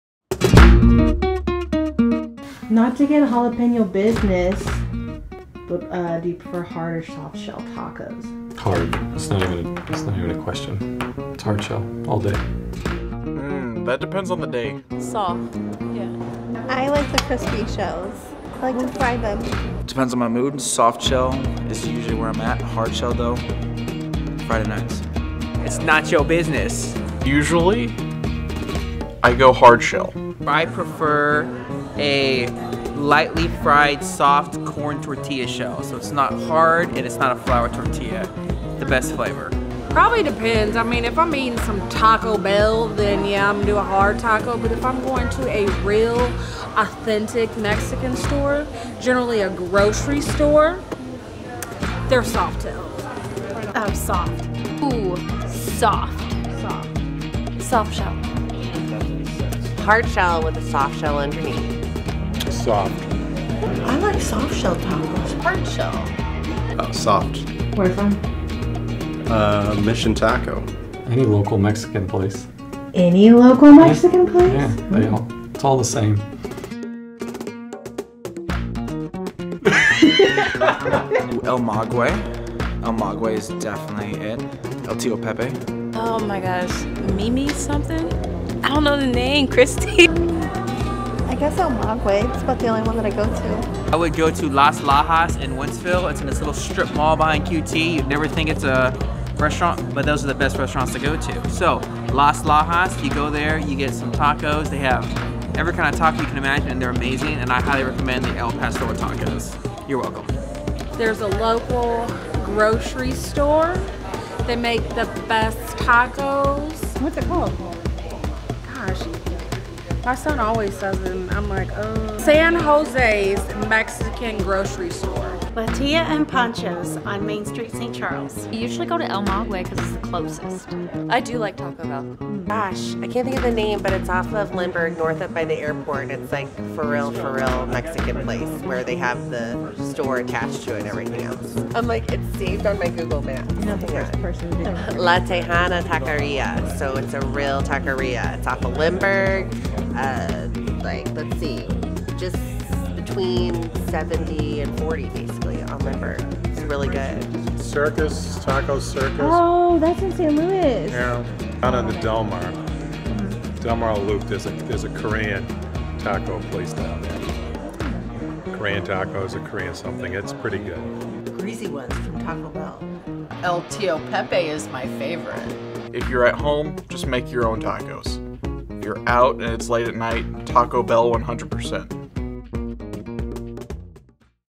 Let's Taco 'Bout It.
Last Taco Tuesday we had some input from your fellow North Star-ians on what and where you should be ordering!